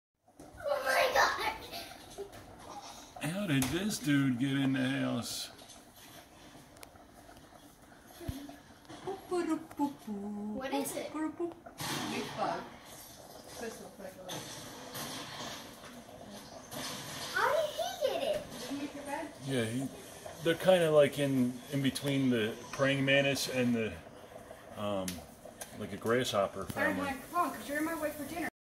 Katydid leaf insect snuck in my house
Katydid, long horned grasshopper, or leaf bug